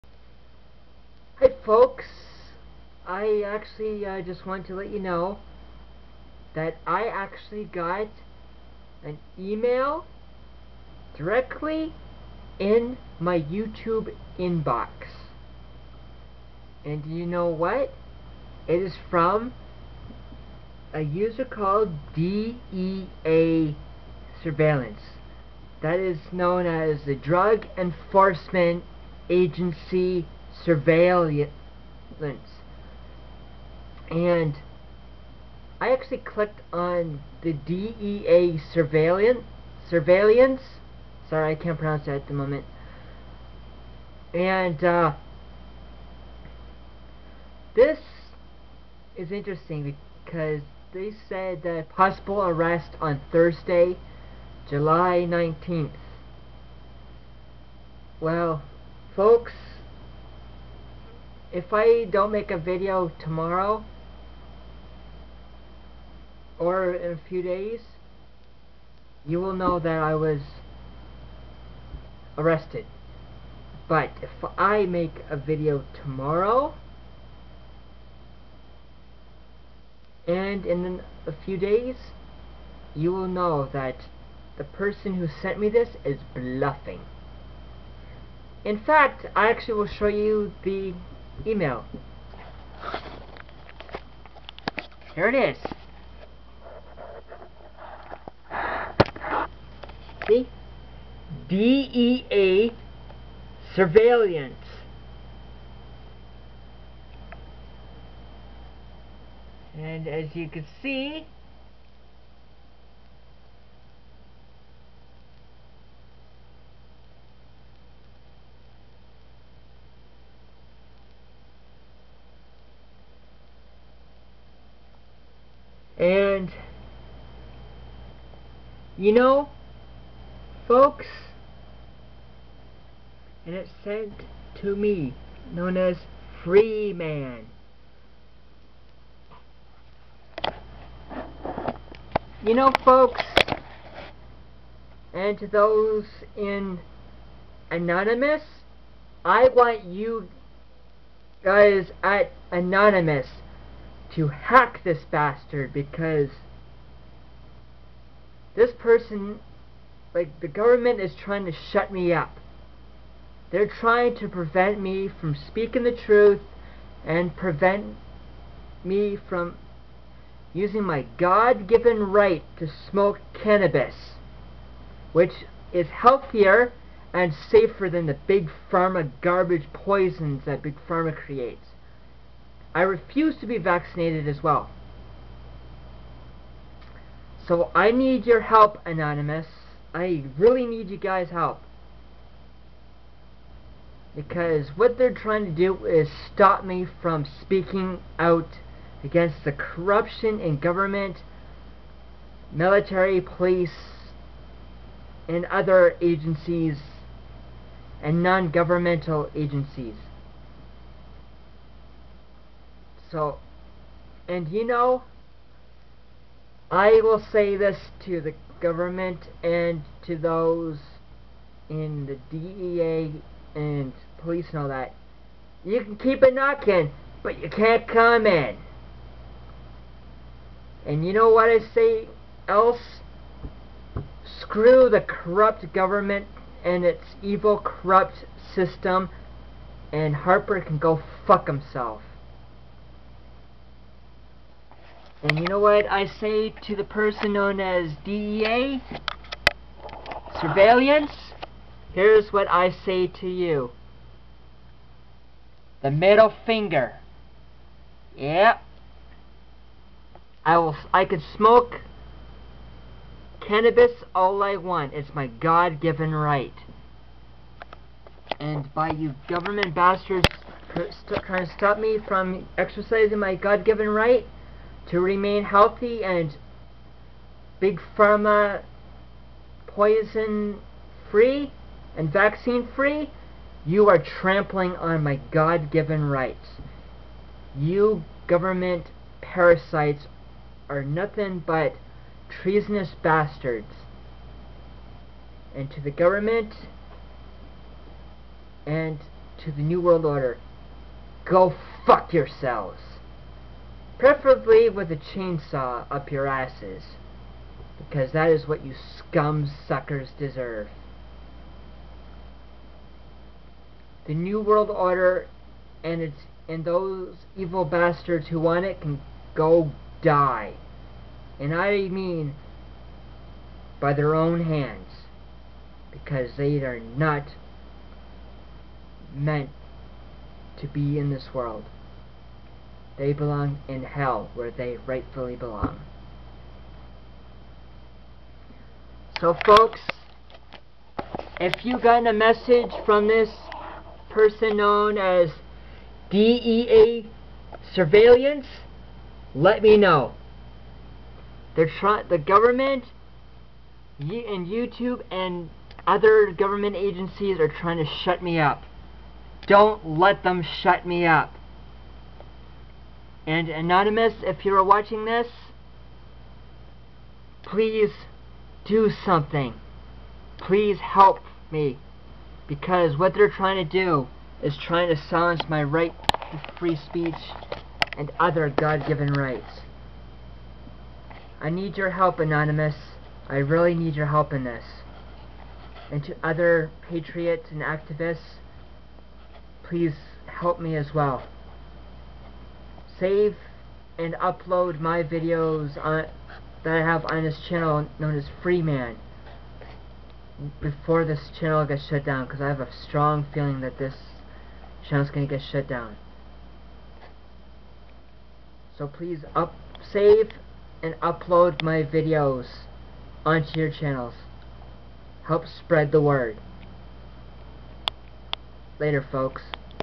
A Messege I Just Got In My Youtube Inbox
You need to see and read this e-mail I got from the DEAsurveillance. Let me know what you think. Have any of you recieved a messege like this before. If so then please comment and do a video response to this video.

Here is the e-mail messege below:

From: DEAsurveillance

Date: 07/18/12

Subject:

Controlled Substances File# 34990/2012

*** WARNING: This document is meant for internal Drug Enforcement Administration & R.C.M.P. correspondence only. If received in error, you are required by law to delete it ***

track & report... lock on YouTube account... Man

Previously on YouTube under another username: "freedomfighterguy1". That account had been terminated due to severe violations of YouTube Community Guidelines. Perpetrator has now illegally opened new account in contravention of YouTube rules. (info received from YouTube admin)

Online admission of Schedule I drug possession & use

dispatch. IP LOCATER...

*** Canadian authorities notified one hour ago. R.C.M.P. state possible arrest of this suspect on Thursday, July 19.

...w2wvH4h /0/

Sent to:  Free Man